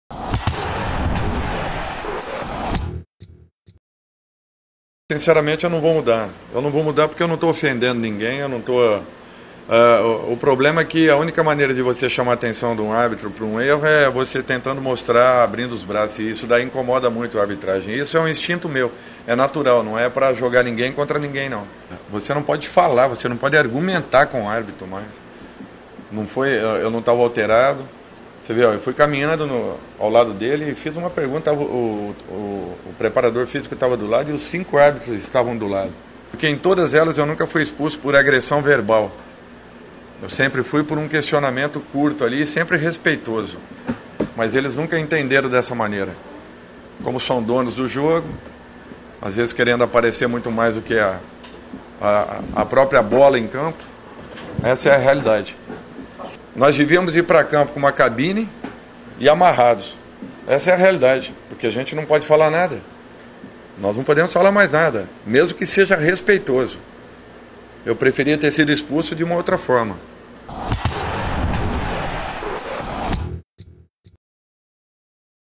Dorival Jr: 'Arbitragem quer aparecer mais do que a bola'
Dorival soltou o verbo e falou tudo sobre o ocorrido diante do
Atlético-MG